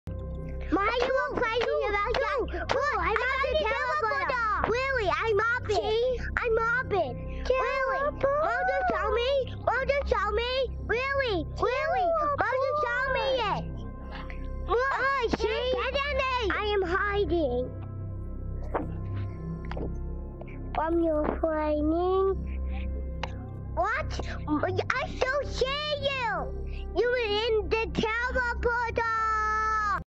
MINDSHOW | Rory doesn't know who you are
Going into a teleporter in the future and being missing.